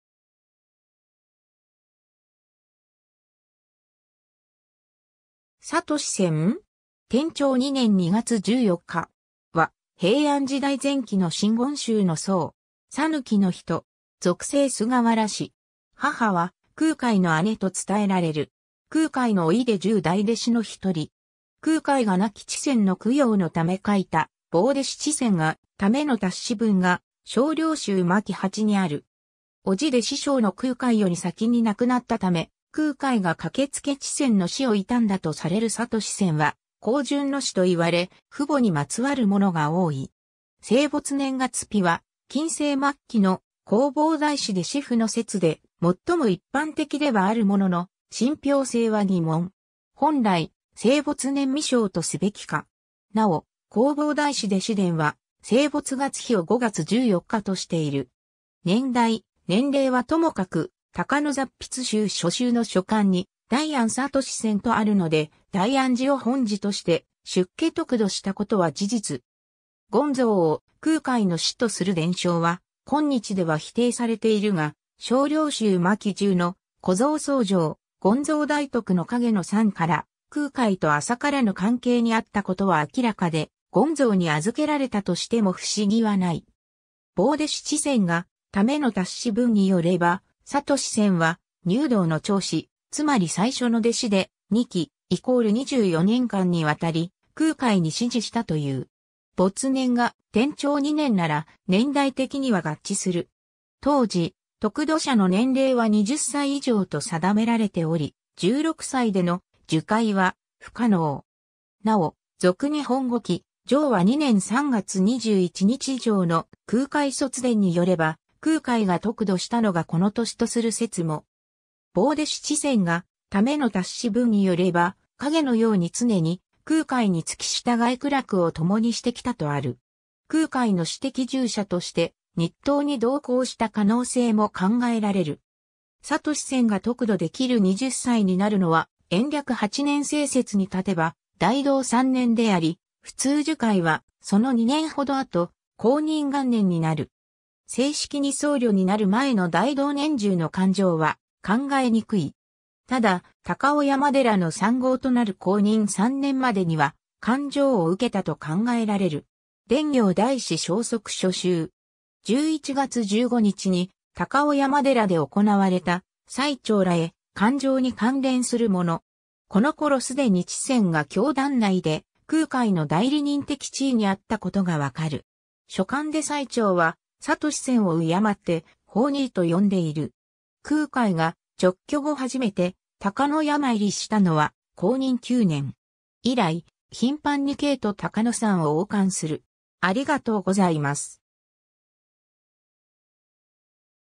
智泉
智泉（ちせん、延暦8年2月14日（789年3月15日）? - 天長2年2月14日（825年3月25日）?）は、平安時代前期の真言宗の僧。
讃岐の人。
俗姓菅原氏（一説に阿刀氏）。
母は空海（弘法大師）の姉と伝えられる。
空海の甥で十大弟子の一人。
空海が亡き智泉の供養のため書いた「亡弟子智泉が為の達嚫文」が『性霊集』巻八にある。
 叔父で師匠の空海より先に亡くなったため、空海が駆けつけ智泉の死を悼んだとされる 智泉は孝順の士といわれ、父母にまつわるものが多い。
 ^ 生没年月日は近世末期の『弘法大師弟子譜』の説で、最も一般的ではあるものの、信憑性は疑問。
本来、生没年未詳とすべきか。
なお、『弘法大師弟子伝』は生没月日を5月14日としている。
 ^ 年代・年齢はともかく、『高野雑筆集』所収の書簡に「大安智泉」とあるので、大安寺を本寺として出家得度したことは事実。
勤操を空海の師とする伝承は、今日では否定されているが、『性霊集』巻十の「故贈僧正勤操大徳の影の讃」から、空海と浅からぬ関係にあったことは明らかで、勤操に預けられたとしても不思議はない。
 ^ 「亡弟子智泉が為の達嚫文」によれば、智泉は「入道の長子」、つまり最初の弟子で、「二紀」=24年間にわたり空海に師事したという。
没年が天長2年（825年）なら年代的には合致する。
 ^ 当時、得度者の年齢は20歳以上と定められており、16歳での受戒は不可能。
なお『続日本後紀』承和2年3月21日（789年4月22日）条の空海卒伝によれば、空海が得度したのがこの年（前年の延暦22年（805年）とする説も）。
 ^ 「亡弟子智泉が為の達嚫文」によれば、影のように常に空海に付き随い苦楽をともにしてきたとある。
空海の私的従者として入唐に同行した可能性も考えられる。
 ^ 智泉が得度できる20歳になるのは、延暦8年（789年）生説に立てば大同3年（808年）であり、ふつう受戒はその2年ほど後、弘仁元年（810年）になる。
正式に僧侶になる前の大同年中の灌頂は考えにくい。
ただ、高雄山寺の三綱となる弘仁3年までには灌頂を受けたと考えられる。
 ^ 『伝教大師消息』所収。
11月15日（12月22日）に高雄山寺で行われた最澄らへ灌頂に関連するもの。
この頃すでに智泉が教団内で空海の代理人的地位にあったことがわかる。
書簡で最澄は智泉を敬って「法兄」と呼んでいる。
 ^ 空海が勅許後はじめて高野山入りしたのは弘仁9年（818年）。
以来、頻繁に京と高野山を往還する。